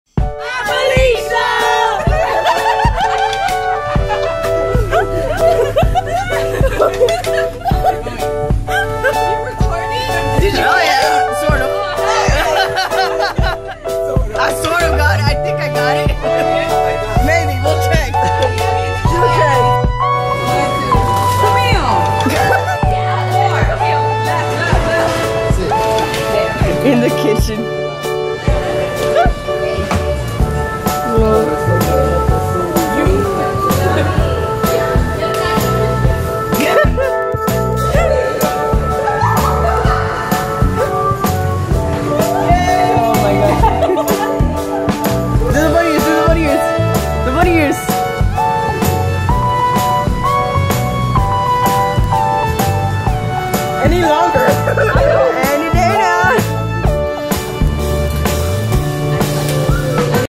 Just a Random DSO Video with friends :)
Hi 16ers this is your Visually Impaired Friend Roldan0716 and I just wanted to post a random video of me and my College friends. So enjoy the video and see you guys soon!! :)